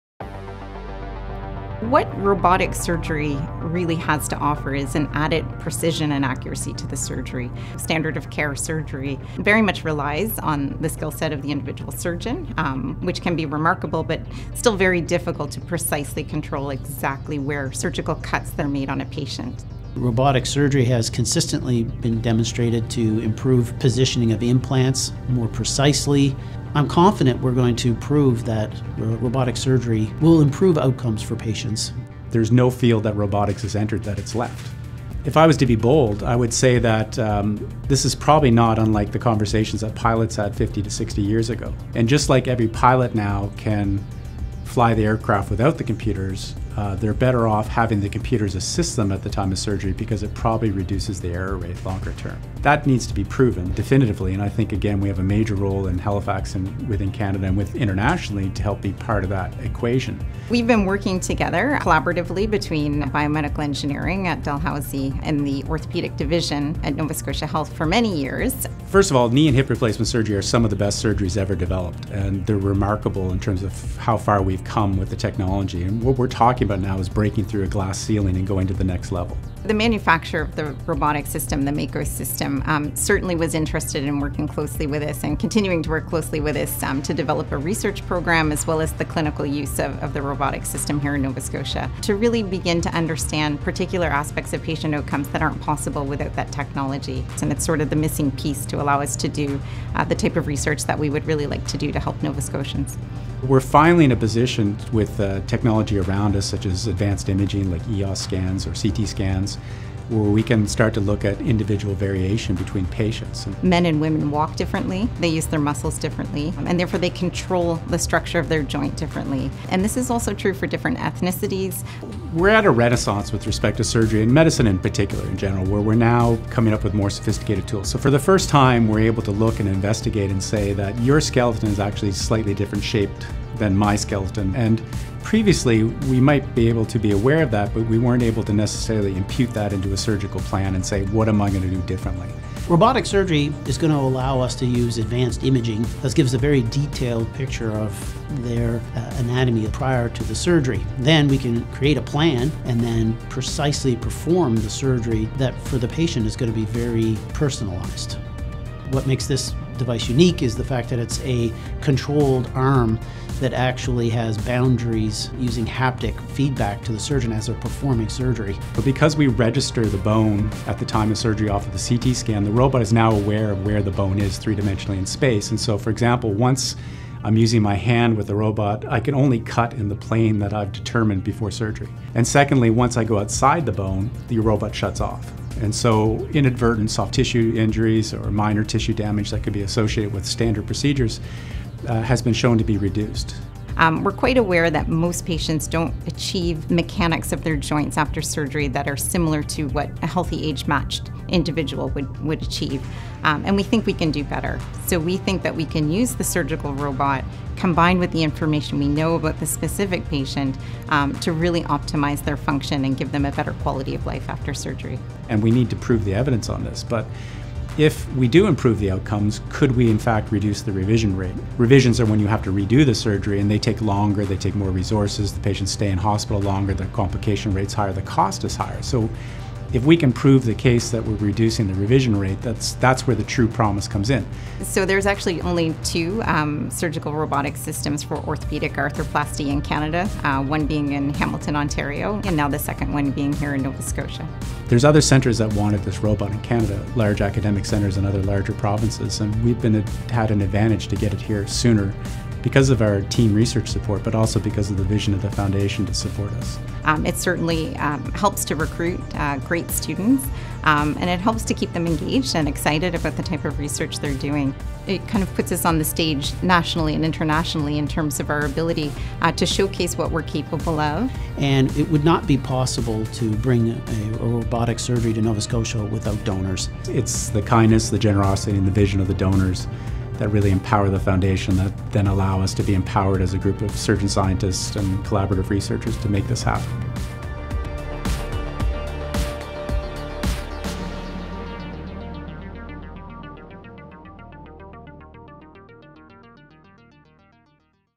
Canada's second orthopaedic surgical robot
We're calling on our donor community to fund Canada's second surgical robot for surgeries of the hip and knee. The QEII seized an opportunity to receive the robot to conduct research that will shape care in the future.